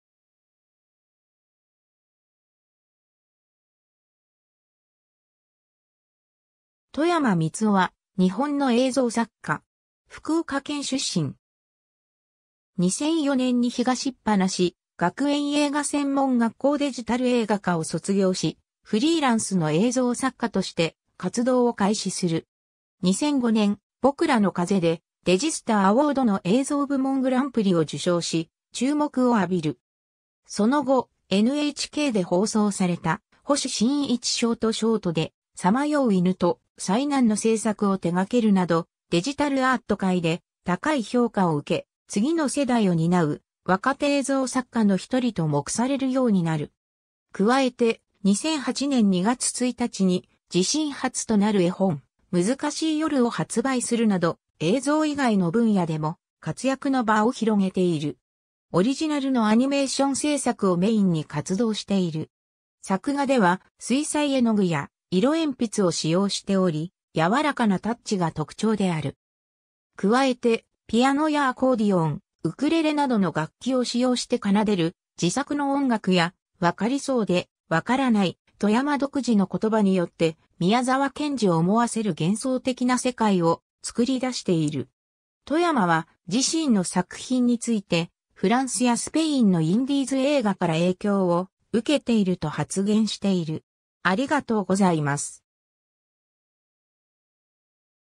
外山光男
デジタル・アーティスト
外山 光男（とやま みつお、1982年 - ）は、日本の映像作家。
 福岡県出身。
2004年に東放学園映画専門学校デジタル映画科を卒業し、フリーランスの映像作家として活動を開始する。
 2005年、『ぼくらの風』でデジスタ･アウォードの映像部門グランプリを受賞し、注目を浴びる。
その後、NHKで放送された『星新一ショートショート』で「さまよう犬」と「災難」の制作を手がけるなど、デジタルアート界で高い評価を受け、次の世代を担う若手映像作家の１人と目されるようになる。
 加えて、2008年2月1日に自身初となる絵本『むずかしい夜』を発売するなど、映像以外の分野でも活躍の場を広げている。
 オリジナルのアニメーション制作をメインに活動している。
 作画では水彩絵具や色鉛筆を使用しており、やわらかなタッチが特徴である。
加えてピアノやアコーディオン、ウクレレなどの楽器を使用して奏でる自作の音楽や、分かりそうで分からない外山独自の言葉によって、宮沢賢治を思わせる幻想的な世界を作り出している。
 外山は自身の作品について、フランスやスペインのインディーズ映画から影響を受けていると発言している。